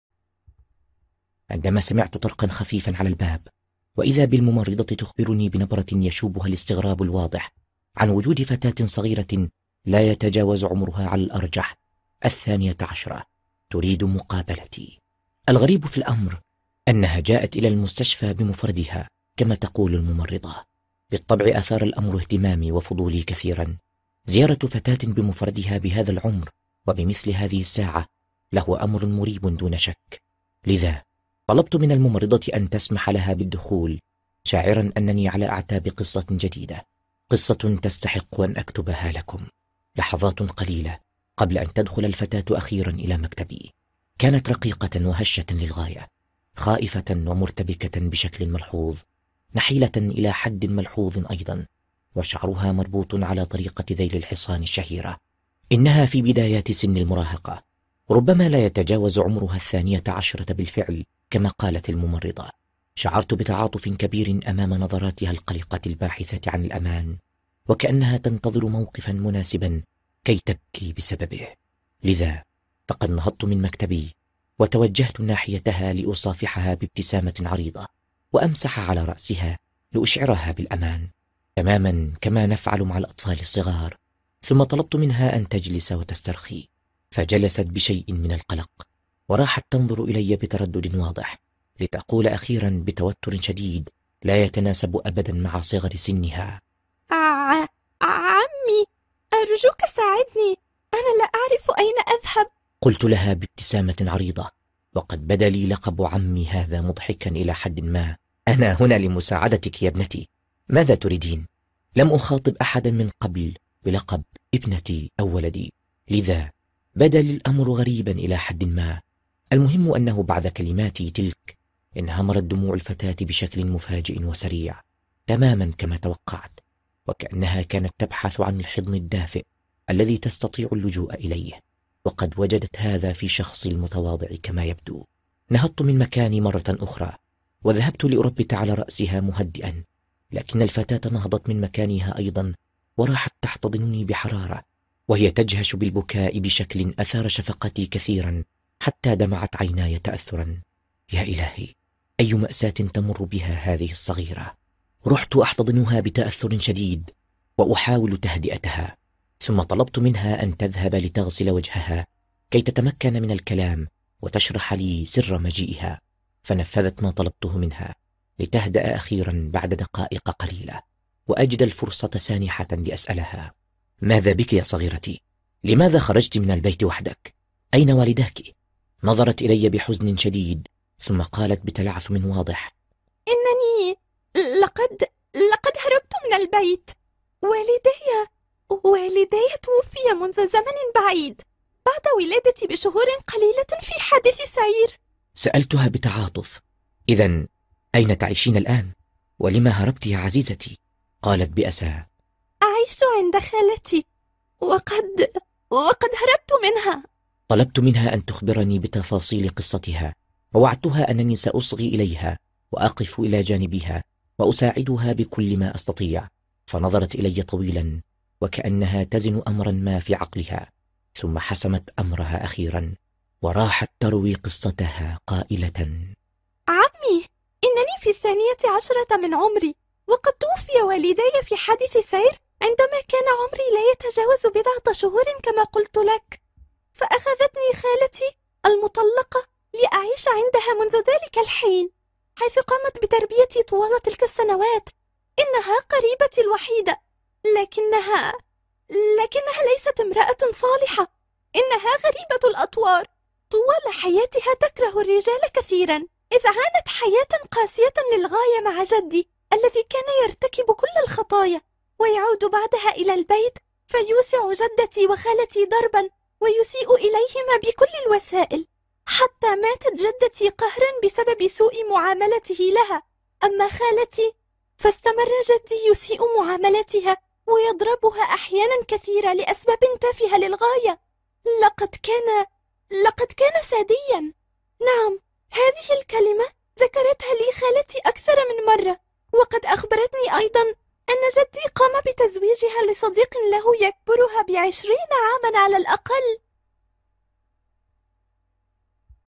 استمع الي رواية حالات نادرة (حمدي التايه وريحانة صوابي)
ما هي مشاكل المراهقات في الكويت ؟! .. هل هي شبيهة بالمشاكل المعتادة التي نتعرض لها جميعا ؟! .. أم أن لهن أسرارا مخيفة لا يعرف عنها الكبار شيئا ؟!! .. أطرح هذا السؤال بسبب عملي كطبيب نفسي .. وبسبب الحالات التي تمر علي في المستشفى .. ففي كل مرة تقريبا تجلس أمامي فتاة في سن المراهقة .. أجدها تروي لي أحداثا مذهلة لا تصدق عن حياتها الشخصية .. وأجد في جعبتها أسرارا سوداء يقشعر لها البدن .. فقررت أن أجمع تلك القصص لأنشرها لكم .. عالما أنها ستجعلكم تفكرون في تفاصيلها حتى بعد الانتهاء منها .. لأننا نتحدث هنا عن حالات غريبة لم يرد معظمها في قاموس الطب النفسي .. حالات نادرة !!!

الراوي
حمدي التايه وريحانة صوابي